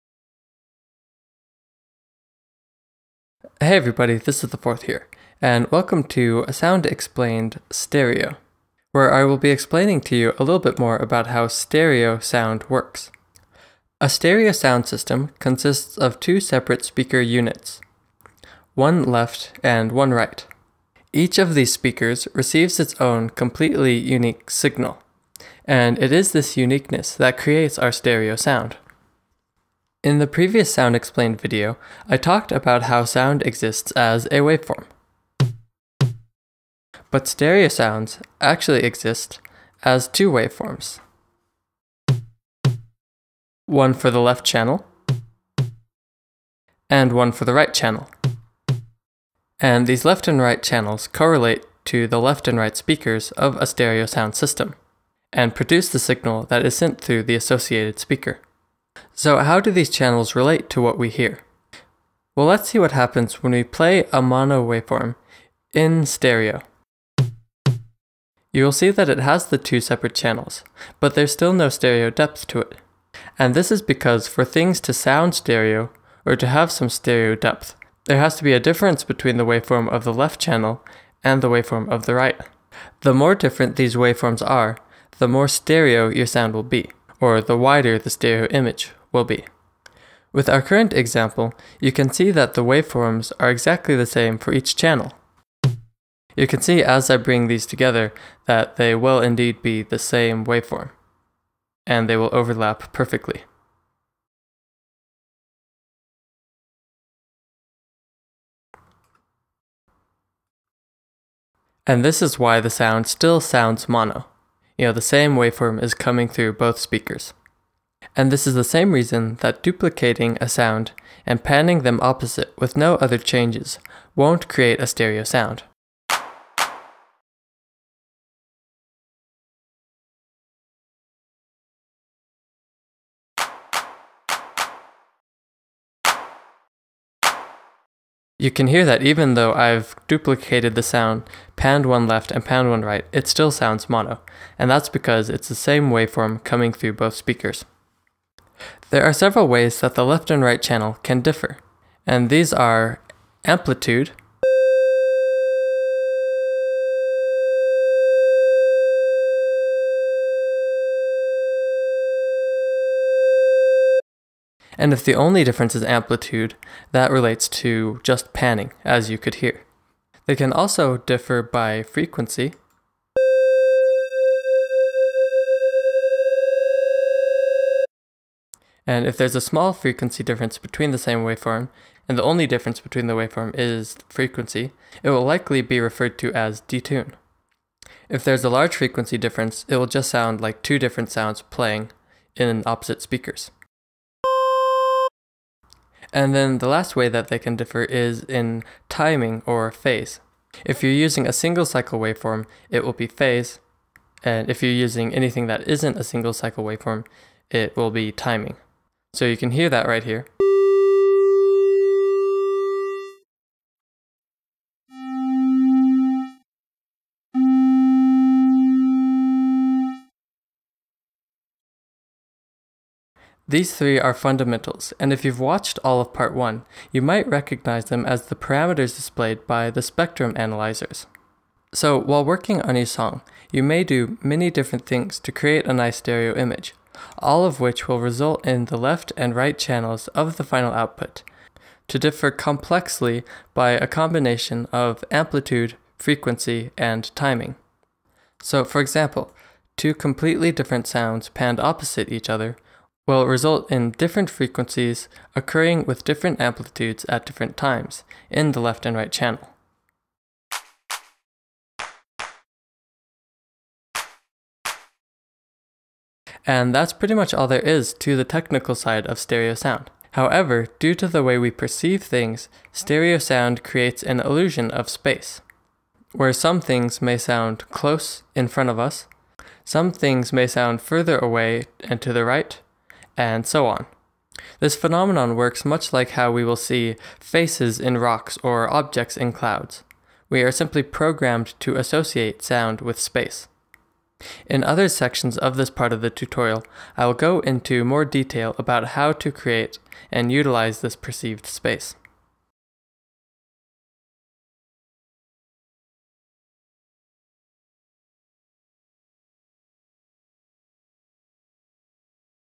Sound Explained: Stereo
In this tutorial video I explain the basics of how stereo sound works.